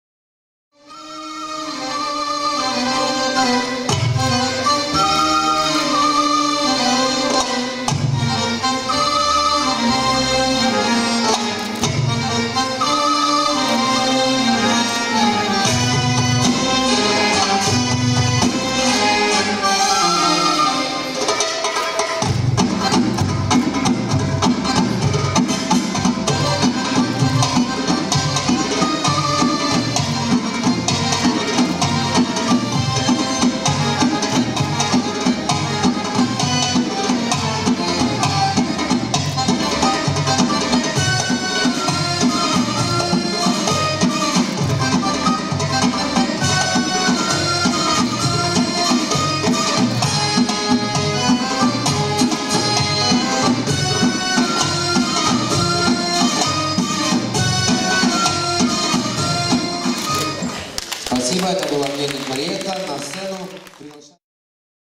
Marietta Melnik ⊰⊱ Primorye CUP '12.
Мариетта Мельник на IV Кубке Приморья «Экспрессия Востока» г. Бердянск, гДК 12.08.12.
(Любители продолжающие полуфинал) .